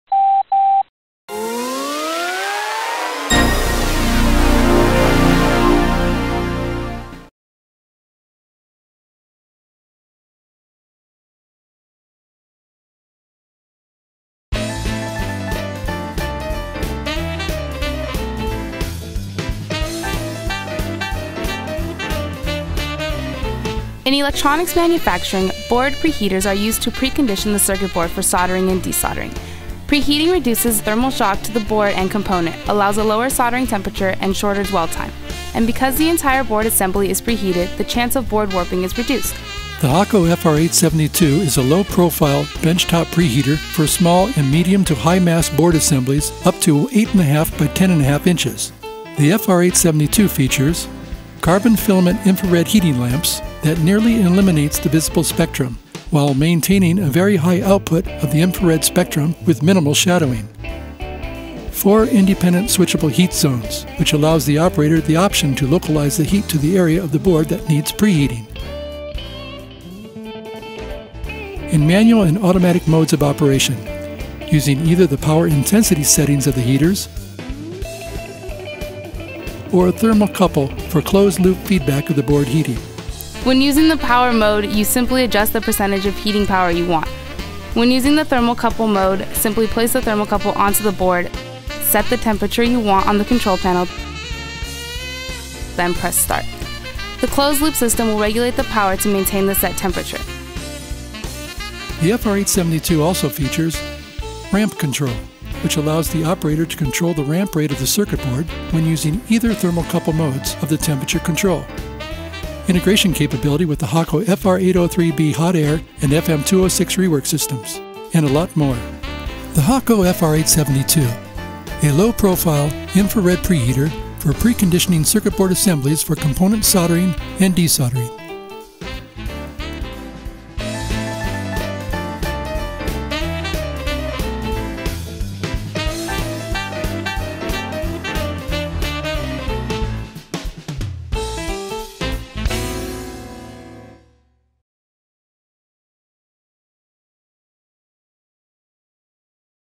Hakko FR-872 IR PCBoard Preheater by American Hakko
This is the official video from American Hakko — The FR-872 is a low profile, bench top preheater designed to elevate the temperature of circuit board assemblies so that components on them can be soldered and de-soldered more easily and with improved results.

This is particularly useful on medium to high-mass board assemblies, which can "heat-sink" a significant amount of energy (heat) being applied by a soldering iron tip or handheld hot air tool.